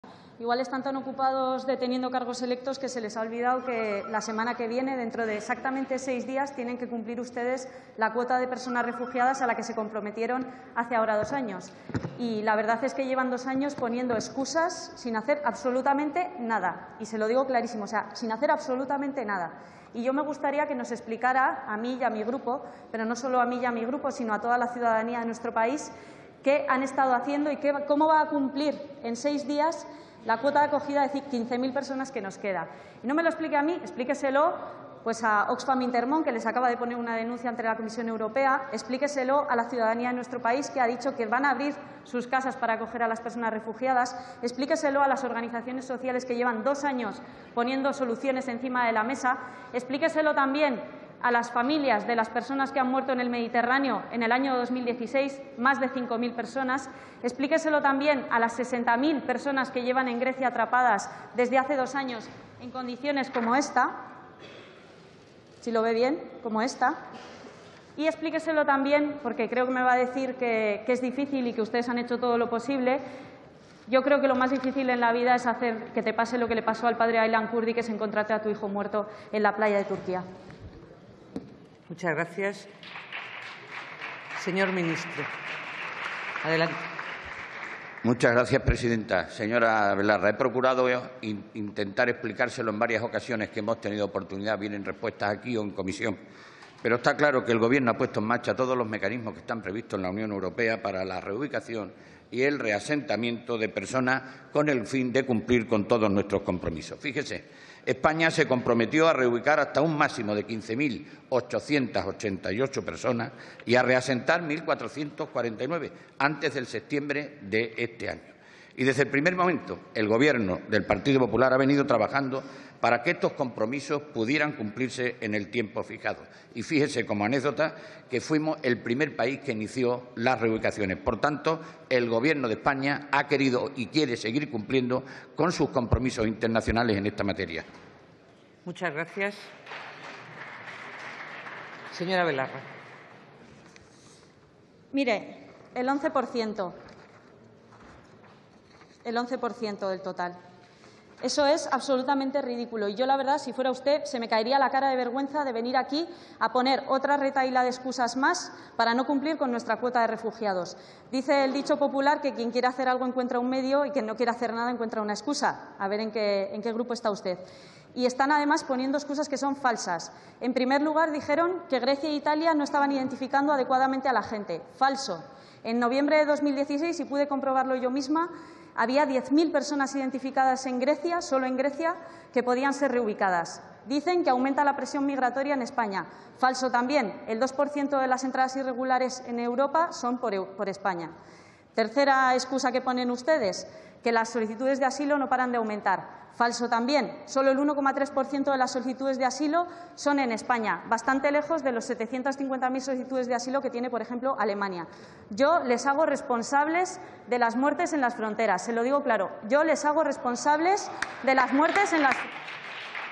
"Solo se ha cumplido el 11% del total de la cuota de asentamiento de personas refugiadas"
Ione Belarra pregunta al Ministro de Interior ¿Qué mecanismos está implementando el Gobierno para cumplir el compromiso de reubicación y asentamiento de 17.383 personas refugiadas según se comprometió en la Decisión UE 2015/1061?